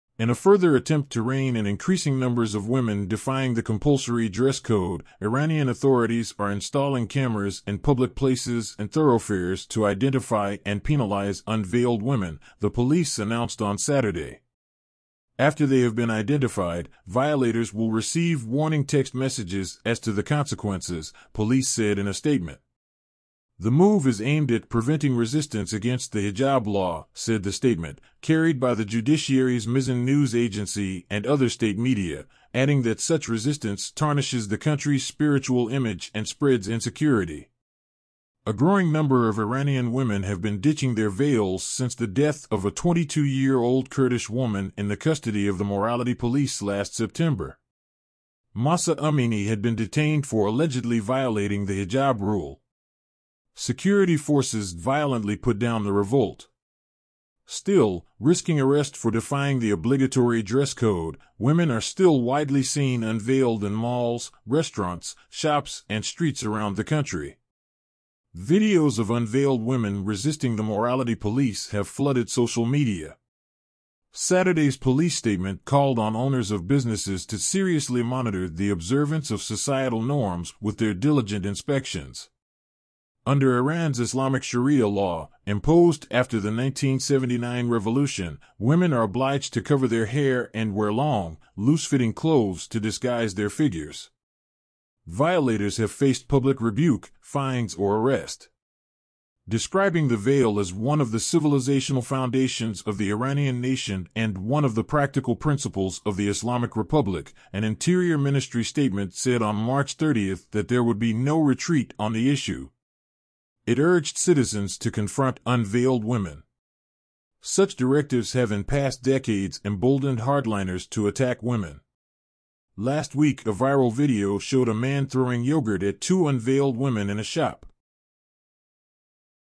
Cameras in Public Places to Identify, Penalise Unveiled Women
Iranian authorities are installing cameras in public places and thoroughfares to identify and penalise unveiled women, the police announced on Saturday.

After they have been identified, violators will receive “warning text messages as to the consequences”, police said in a statement.

The move is aimed at “preventing resistance against the hijab law,” said the statement, carried by the judiciary’s Mizan news agency and other state media, adding that such resistance tarnishes the country’s spiritual image and spreads insecurity.

A growing number of Iranian women have been ditching their veils since the death of a 22-year-old Kurdish woman in the custody of the morality police last September. Mahsa Amini had been detained for allegedly violating the hijab rule. Security forces violently put down the revolt.